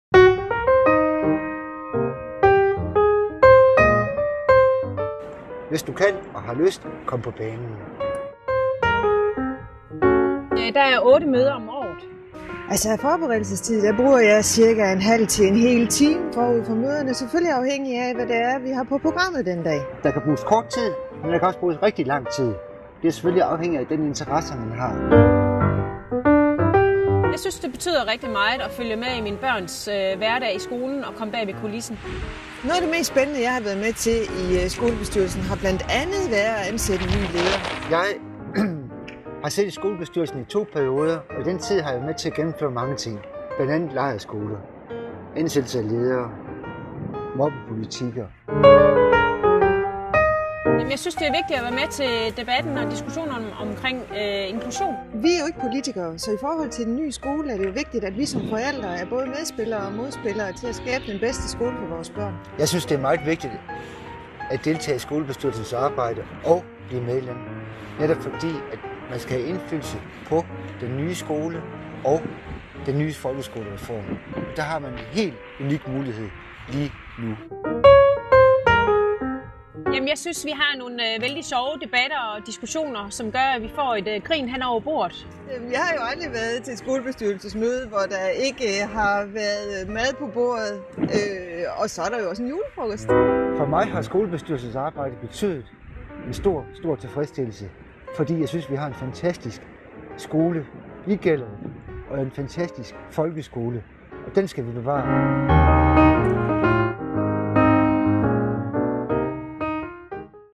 Skolebestyrelsen Gjellerupskole ved Herning
Har du 2 minutter?
Mød i videoen tre forældrerepræsentanter fra skolebestyrelsen på Gjellerupskole 2014.
Der er 7 forældre i bestyrelsen. Tre af dem stiller ikke op igen, så der er plads til dig, som vil gøre en indsats.
Valget er den 8. april 2014, og du kan melde dig som kandidat inden den 25. marts.